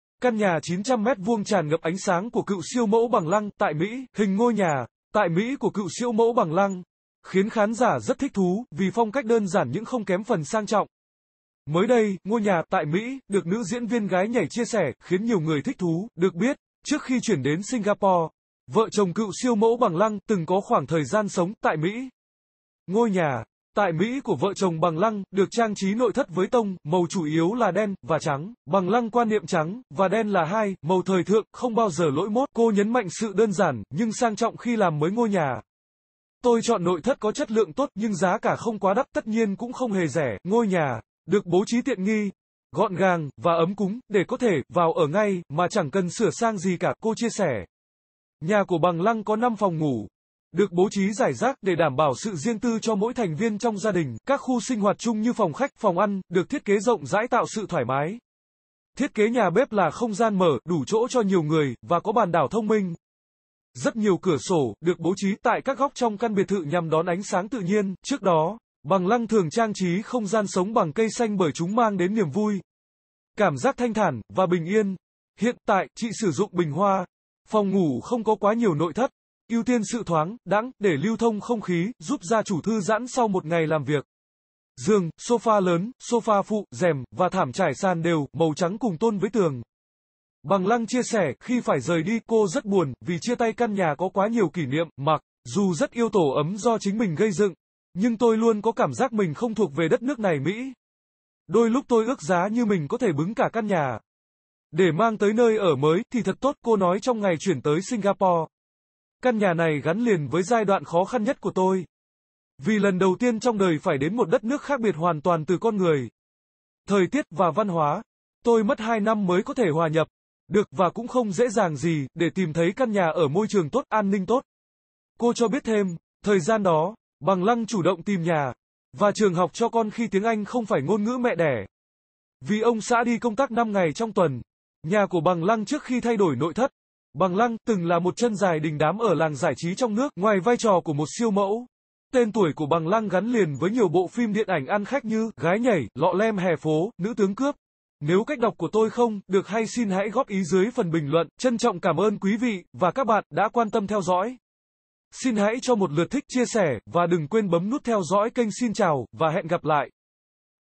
Căn nhà 900m2 tràn ngập ánh sáng của cựu siêu mẫu Bằng Lăng tại Mỹ
Siêu sale Sen Đỏ dành cho bạn Đăng kí kênh xem tin tức mới nhất Hình ngôi nhà tại Mỹ của cựu siễu mẫu Bằng Lăng khiến khán giả rất thích thú vì phong cách đơn giản những không kém phần sang trọng.Mới đây, ngôi nhà tại Mỹ được nữ diễn viên "Gái nhảy" chia sẻ khiến nhiều người thích thú
Được biết, trước khi chuyển đến Singapore, vợ chồng cựu siêu mẫu Bằng Lăng từng có khoảng thời gian sống tại Mỹ.Ngôi nhà tại Mỹ của vợ chồng Bằng Lăng được trang trí nội thất với tông màu chủ yếu là đen và trắng
Bằng Lăng quan niệm trắng và đen là hai màu thời thượng, không bao giờ lỗi mốt
Cô nhấn mạnh sự đơn giản nhưng sang trọng khi làm mới ngôi nhà.'Tôi chọn nội thất có chất lượng tốt nhưng giá cả không quá đắt, tất nhiên cũng không hề rẻ
Ngôi nhà được bố trí tiện nghi, gọn gàng và ấm cúng để có thể vào ở ngay mà chẳng cần sửa sang gì cả', cô chia sẻ.Nhà của Bằng Lăng có 5 phòng ngủ được bố trí rải rác để đảm bảo sự riêng tư cho mỗi thành viên trong gia đình
Các khu sinh hoạt chung như phòng khách, phòng ăn được thiết kế rộng rãi tạo sự thoải mái.Thiết kế nhà bếp là không gian mở, đủ chỗ cho nhiều người và có bàn đảo thông minh.Rất nhiều cửa sổ được bố trí tại các góc trong căn biệt thự nhằm đón ánh sáng tự nhiên
Trước đó, Bằng Lăng thường trang trí không gian sống bằng cây xanh bởi chúng mang đến niềm vui, cảm giác thanh thản và bình yên
Hiện tại, chị sử dụng bình hoa.Phòng ngủ không có quá nhiều nội thất, ưu tiên sự thoáng đãng để lưu thông không khí, giúp gia chủ thư giãn sau một ngày làm việc.Giường, sofa lớn, sofa phụ, rèm và thảm trải sàn đều màu trắng cùng tone với tường.Bằng Lăng chia sẻ, khi phải rời đi, cô rất buồn vì chia tay căn nhà có quá nhiều kỷ niệm
'Mặc dù rất yêu tổ ấm do chính mình gây dựng nhưng tôi luôn có cảm giác mình không thuộc về đất nước này (Mỹ)
Đôi lúc tôi ước giá như mình có thể 'bứng' cả căn nhà để mang tới nơi ở mới thì thật tốt', cô nói trong ngày chuyển tới Singapore.'Căn nhà này gắn liền với giai đoạn khó khăn nhất của tôi vì lần đầu tiên trong đời phải đến một đất nước khác biệt hoàn toàn từ con người, thời tiết và văn hóa
Tôi mất hai năm mới có thể hòa nhập được và cũng không dễ dàng gì để tìm thấy căn nhà ở môi trường tốt, an ninh tốt..', cô cho biết thêm
Thời gian đó, Bằng Lăng chủ động tìm nhà và trường học cho con khi tiếng Anh không phải ngôn ngữ mẹ đẻ vì ông xã đi công tác 5 ngày trong tuần.Nhà của Bằng Lăng trước khi thay đổi nội thất.Bằng Lăng từng là một chân dài đình đám ở làng giải trí trong nước
Ngoài vai trò của một siêu mẫu, tên tuổi của Bằng Lăng gắn liền với nhiều bộ phim điện ảnh ăn khách như: Gái nhảy, Lọ lem hè phố, Nữ tướng cướp..